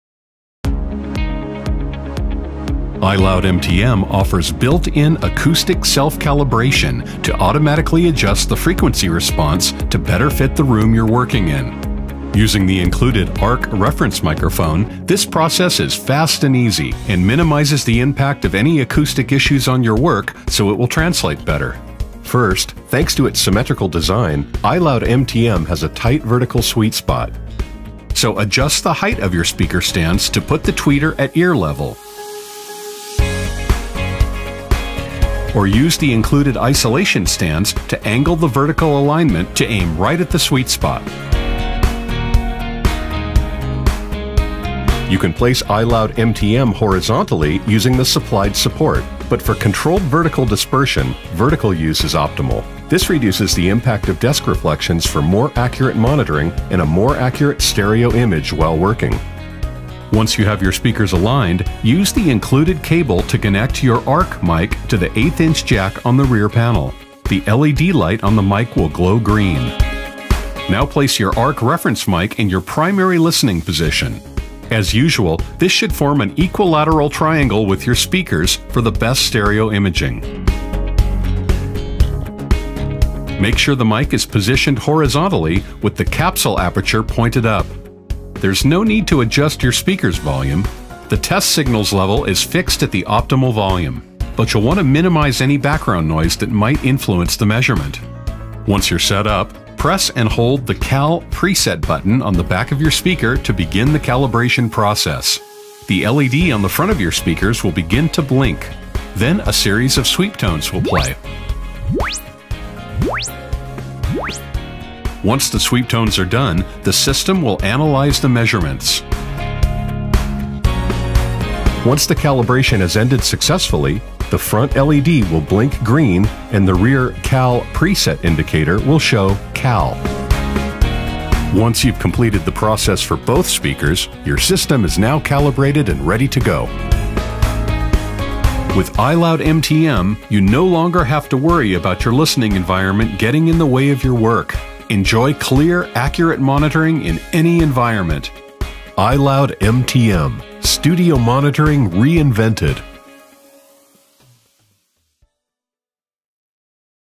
[한글자막] iLoud MTM 스피커 칼리브레이션 방법
동영상을 통해 iLoud MTM 스튜디오 모니터를 빠르고 쉽게 칼리브레이션 하는 방법을 보실 수 있습니다. 스피커에 포함 된 마이크 연결 및 배치, 빠른 칼리브레이션 프로세스 실행까지 단 몇 분 만에 믹싱부터 마스터링을 위한 스피커 설정을 볼 수 있습니다.  한국 공식 수입처 '삼익악기' 의 온라인 스토어 에서 이 뛰어난 모니터 스튜디오 모니터에 대해 보다 자세히 알아보세요.